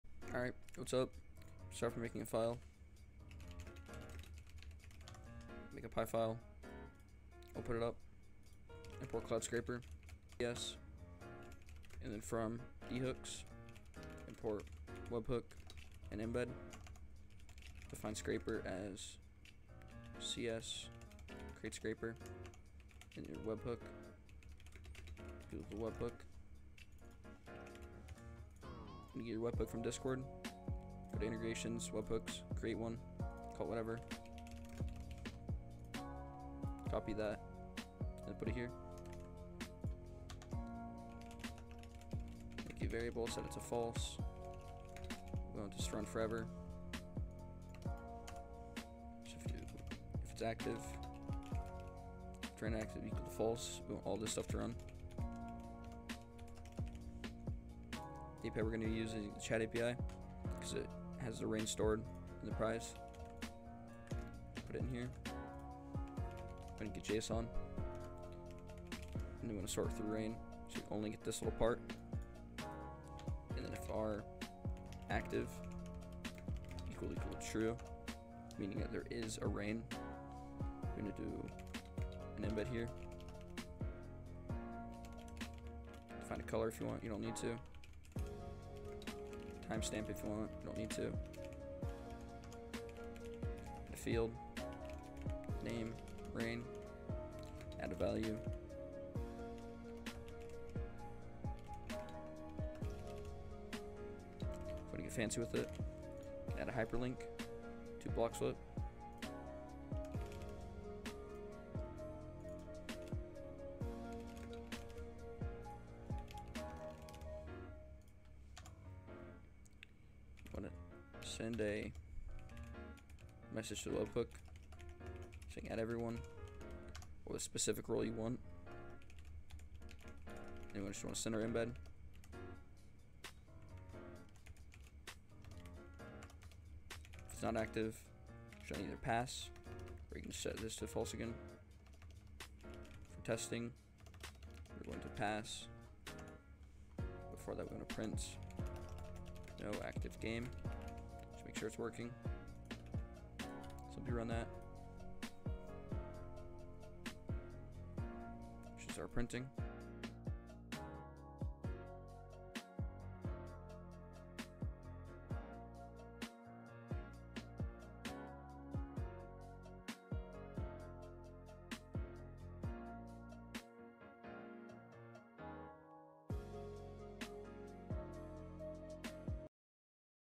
How to make a Bloxflip Rain Noti bot [READ DESCRIPTION]
video how to make a bloxflip rain noti discord bot.
just a simple fun bot to add to your server for more activity.

Also in your cmd type
pip install cloudscraper dhooks
and enter to work
i messed up code a bit so below is correct src
src:
import cloudscraper as cs
from dhooks import Webhook, Embed
from time import sleep

scraper = cs.create_scraper()
webhook = Webhook("webhook link here")

while True:
    rain_active = False
    if rain_active == False:
        if r['active'] == True:
            prize = r['prize']
            em = Embed(color=0x0025ff, timestamp='now')
            em.set_timestamp
            webhook.send("@everyone")
            webhook.send(embed=em)
            rain_active = True
        else:
            rain_active = False